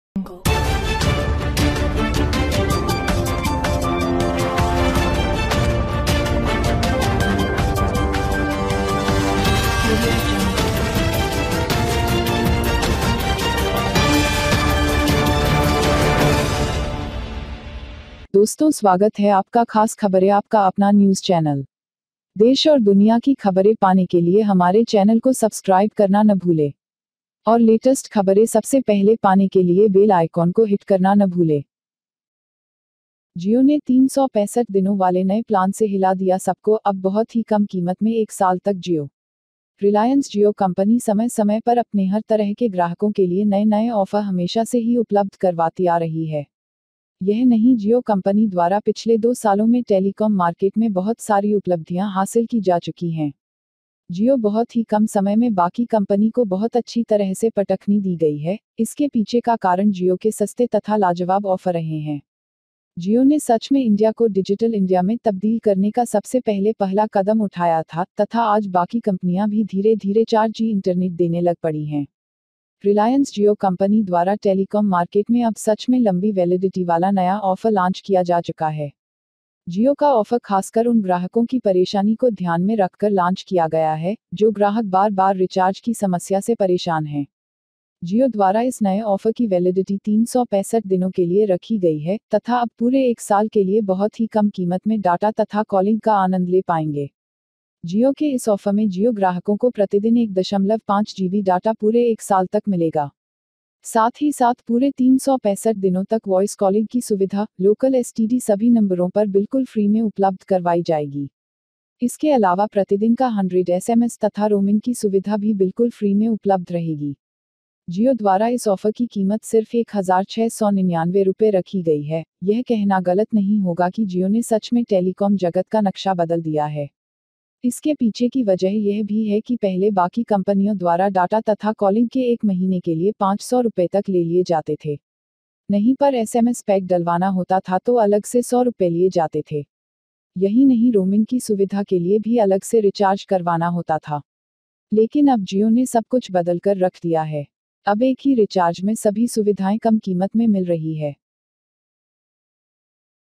जियो ने 365 दिनों वाले नये प्लान से हिला दिया सबको, अब बहुत ही कम कीमत में 1 साल तक जियो
जियो ने 365 दिनों वाले नये प्लान से हिला दिया सबको, अब बहुत ही कम कीमत में 1 साल त